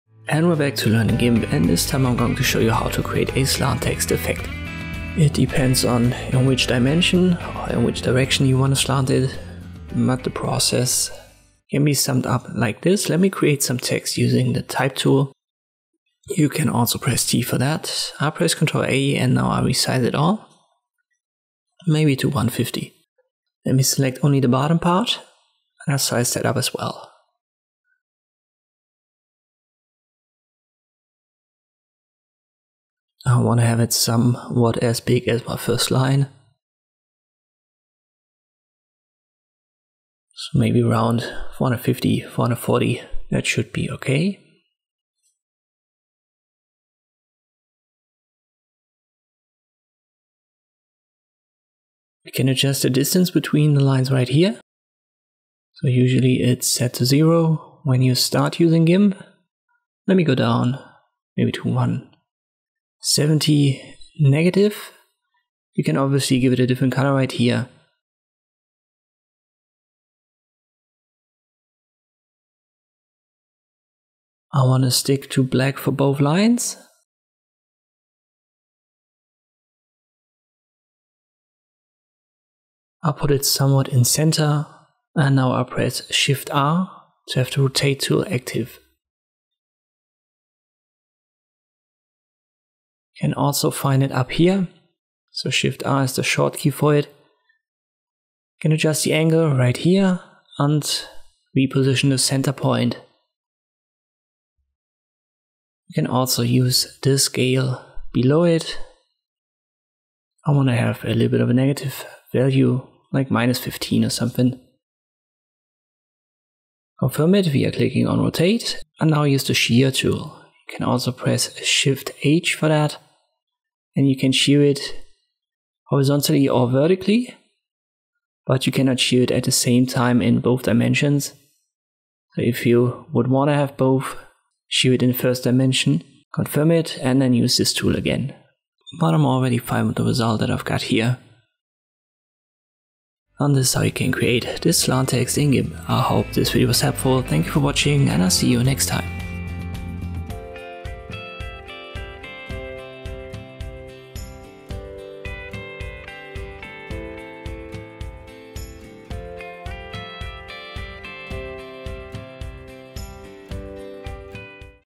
How to Create a Slant Text Effect in Gimp
In this tutorial I'll show you how to create a slant text effect in Gimp.